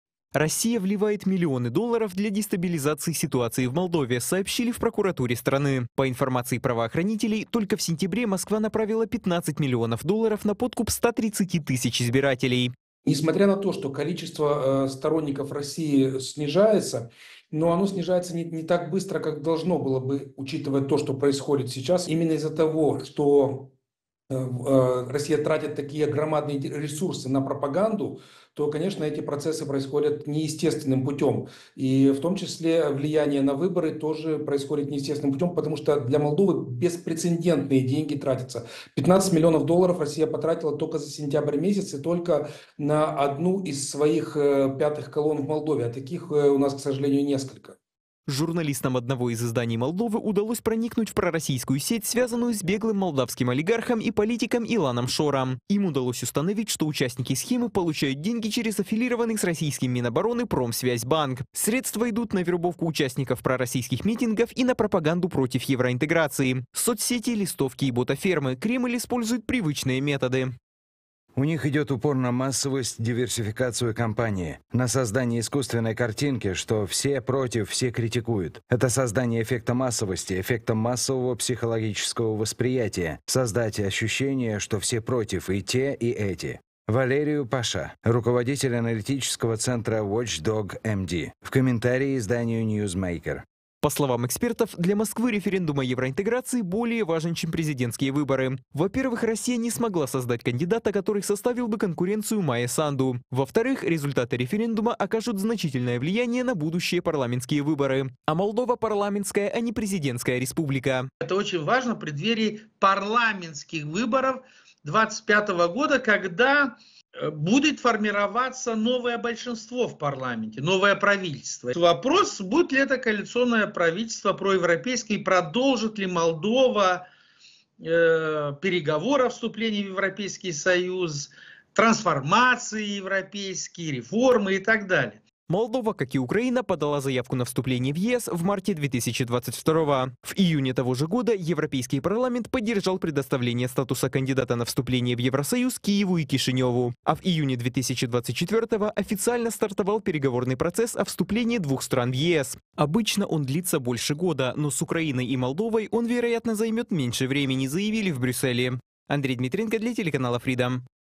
❗ ПУТИН АТАКУЕТ МОЛДОВУ! Пропаганда Кремля просочилась И ТУДА?! ВЫБОРЫ СОРВАНЫ?!
Путин не останавливается только на РФ. Он решил добраться со своей пропагандой и до Молдовы. Его цель — сорвать выборы. Получится ли у диктатора осуществить свой план? Какая ситуация в Молдове? Повлияет ли результат выборов на войну в Украине? Все детали смотрите в нашем видео!